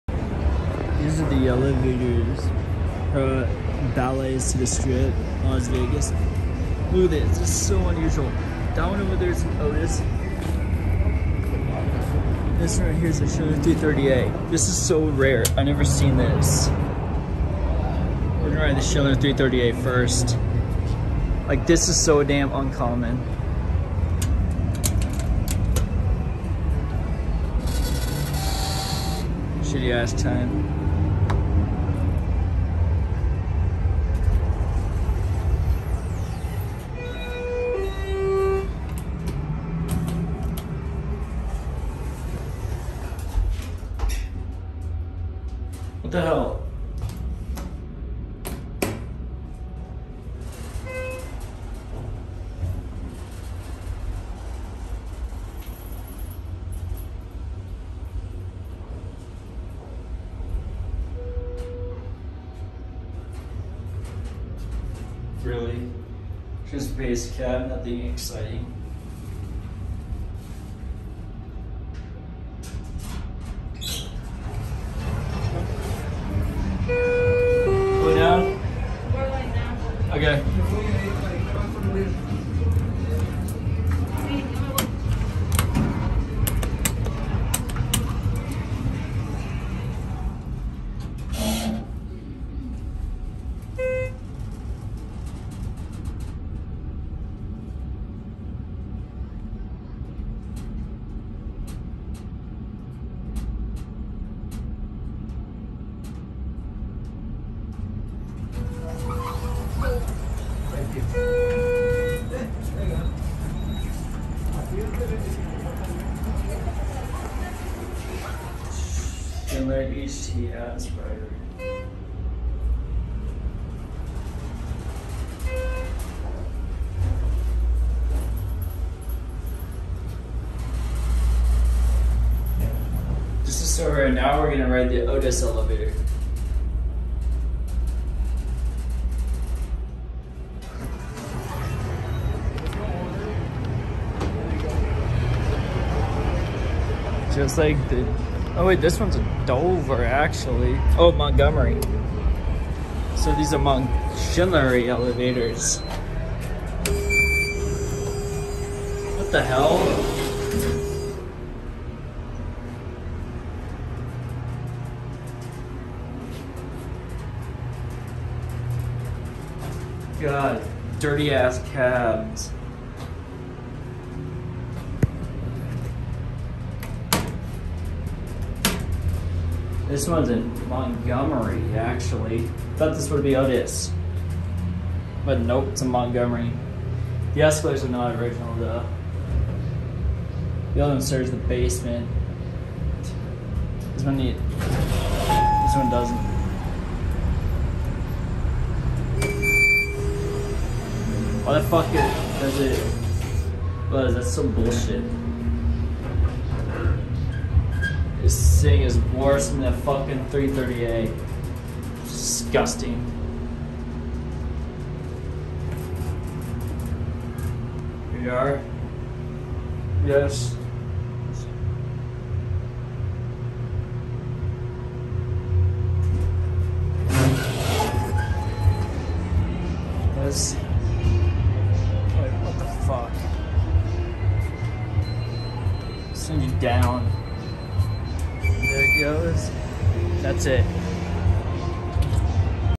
INTERESTING SETUP!! MontSchindlery 330A Hydraulic Elevators at the Bally's Bridge Strip
(September 5, 2020) Las Vegas, Nevada. Apologies for assuming that the other Elevator was an Otis but after the Schindler 330A, I walked over there and saw an Innovation button and then ohhhhhhh!! It's a Montgomery actually!! But this is a VERY UNIQUE setup!! But the reason they added on the 330A is to serve the Basement and I bet this Model had a cheaper bid
Installed: Montgomery 1990-94
Installed: Schindler 2010's